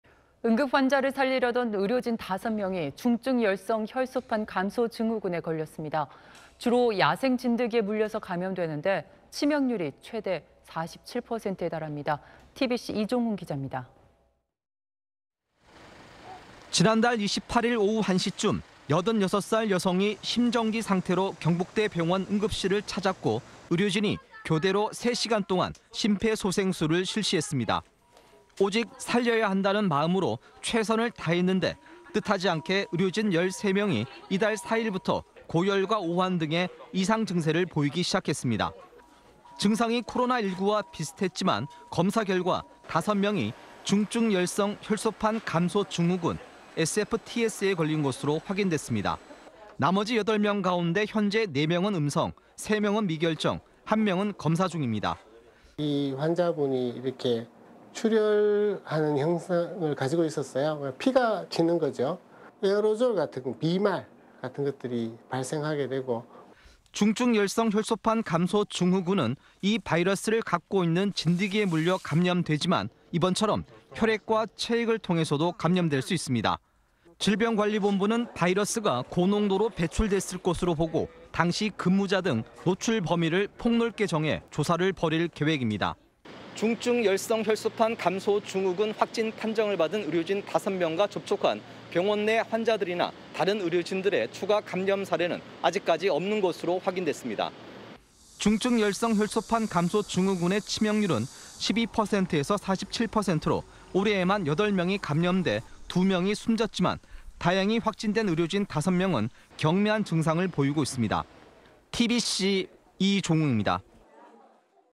'응급환자 심폐소생' 의료진 5명 야생 진드기병 감염 / SBS
응급환자를 살리려던 의료진 5명이 중증열성혈소판감소증후군에 걸렸습니다. 주로 야생 진드기에 물려서 감염되는데, 치명률이 최대 47%에 달합니다.
TBC 이종웅 기자입니다.